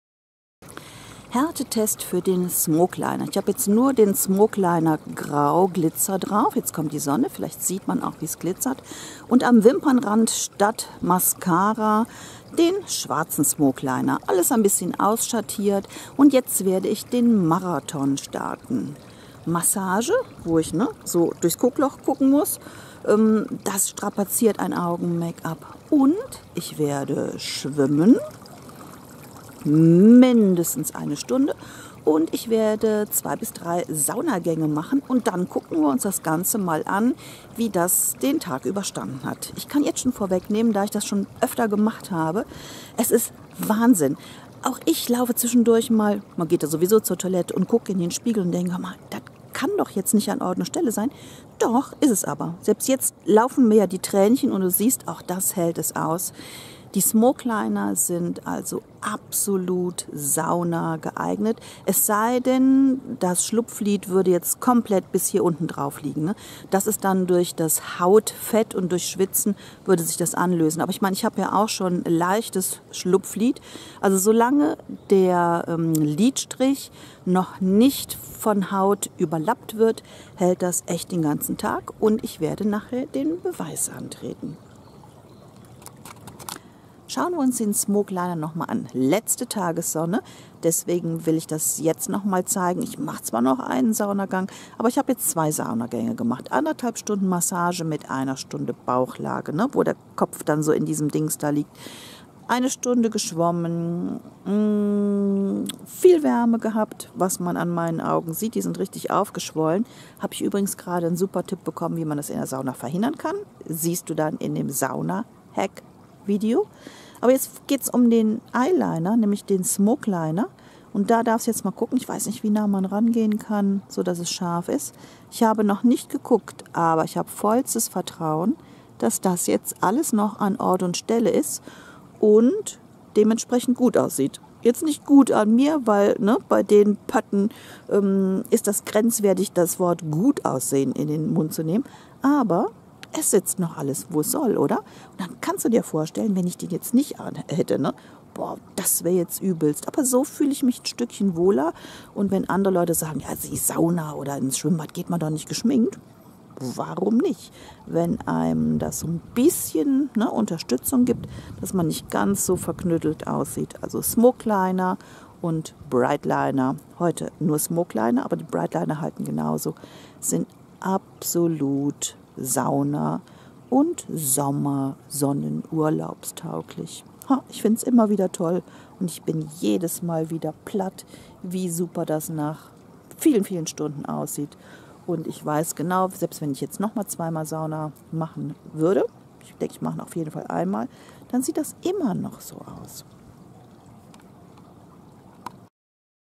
Härtetest: Smokeliner Silbergrau -nach Massage, einer Stunde Schwimmen und 2 Saunagängen
Du brauchst ein richtig lang haltbares Augen Makeup, was Dich nicht im Stich lässt, wenn es auch mal warm, sehr warm wird !?
Dann wirst Du den Smokeliner "Silbergrau" lieben.